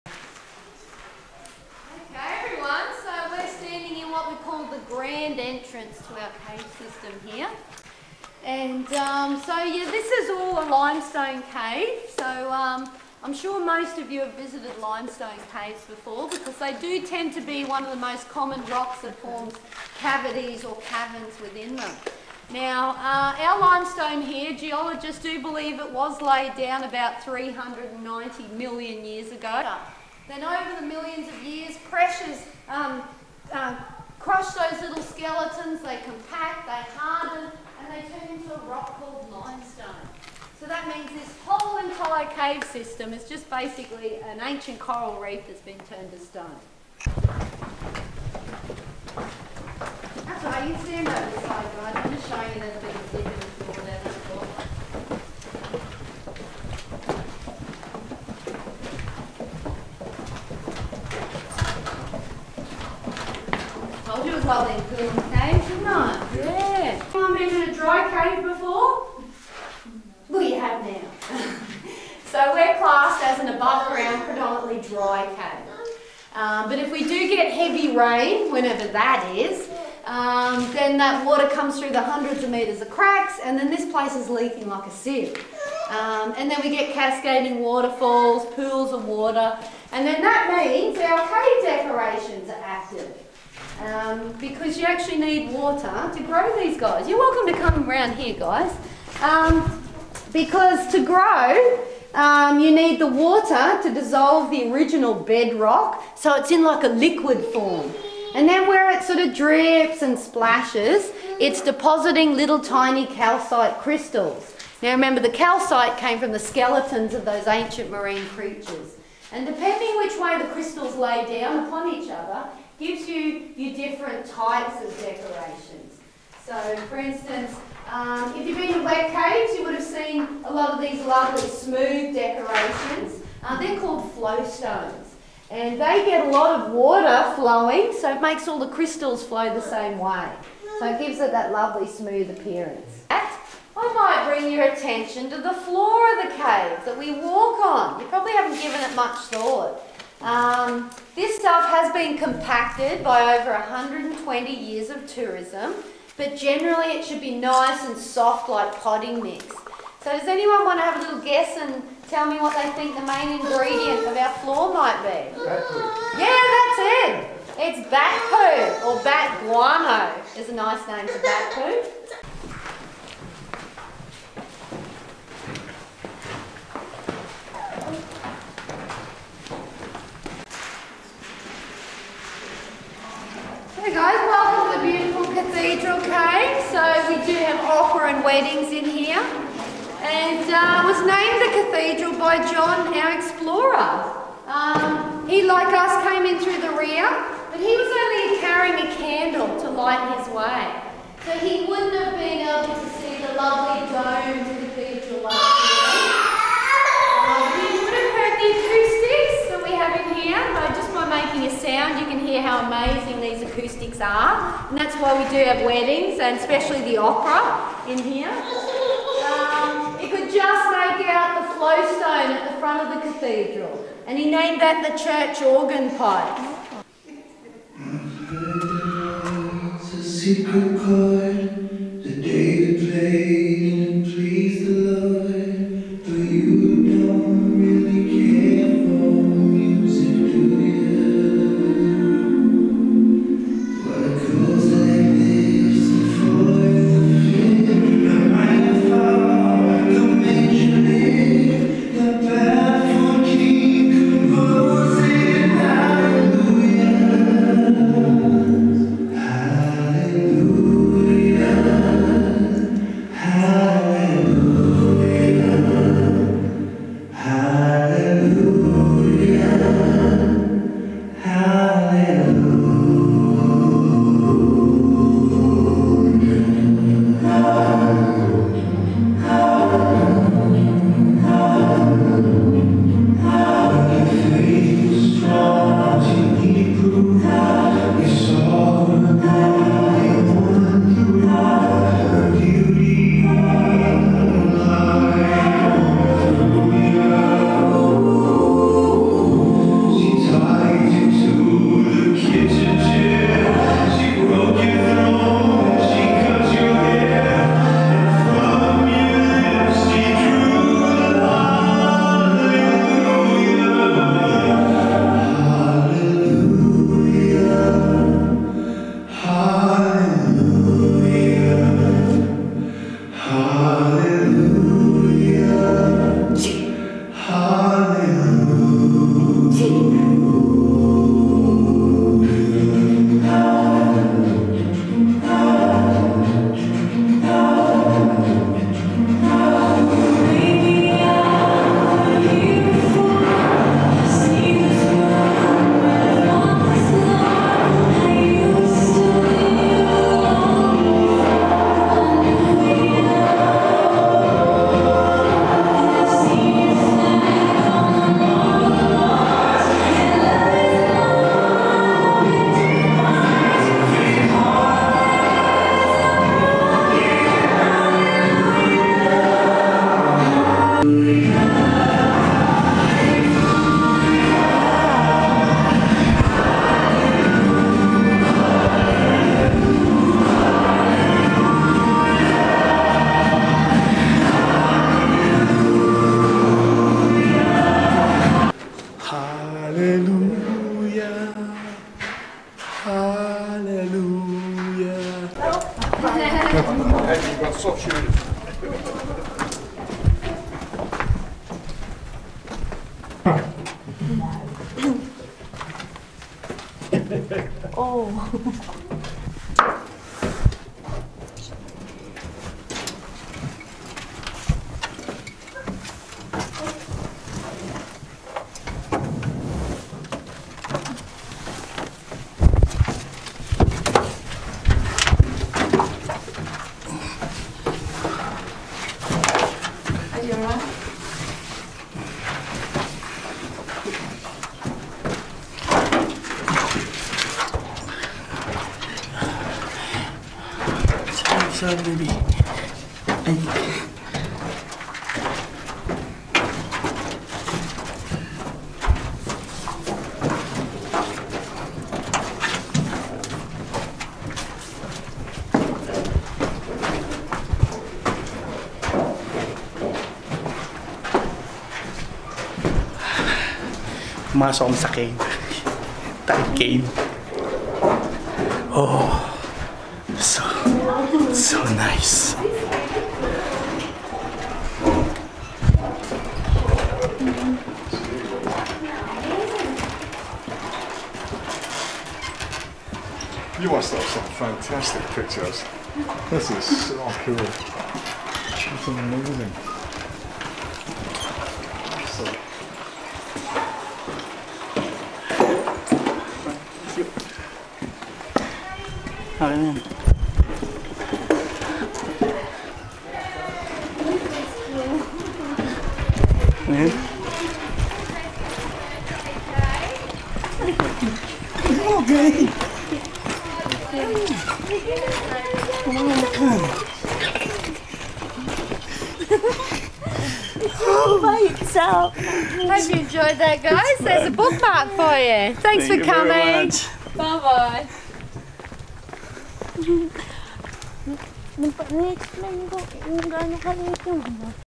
The CAve at Rockhampton
One tourist destination in Queensland Australia, The Cave . spend one hour with the tour guide to see one natures wonder.